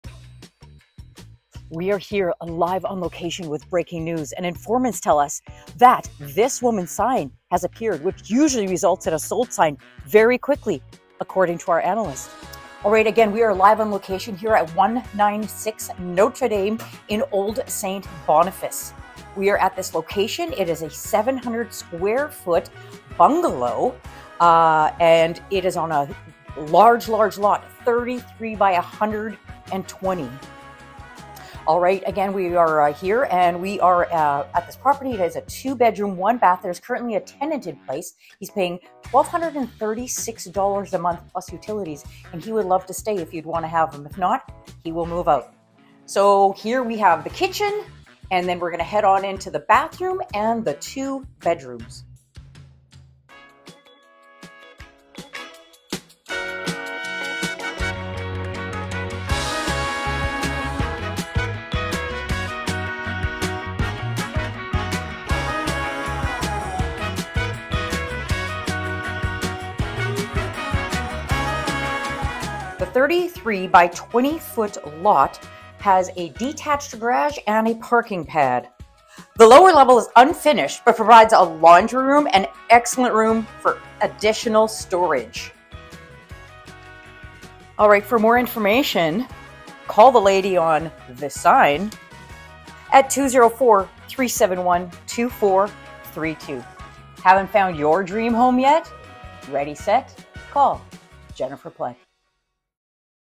196 notre dame
WELCOME HOME to this fantastic 700 sf, 2 bedroom, 1 bath home in Old St Boniface! Large open concept main floor is freshly painted with modern colours throughout. Front foyer, living room and kitchen with all appliances, tile backsplash and laminate flooring. Two good sized bedrooms as well as an updated bathroom complete the main floor. Lower level is unfinished but provides a laundry area and excellent room for additional storage. The backyard is very nice and LARGE ! 33x120 features a garden planter, parking pad, and single car garage. Minutes away from all the great restaurants and shops on Provencher, St B Hospital, Universite de Saint Boniface, Whittier Park for Festival du Voyageur, Downtown shopping and The Forks! This location puts you right in the heart of all the action! current tenant paying $1236/month plus utility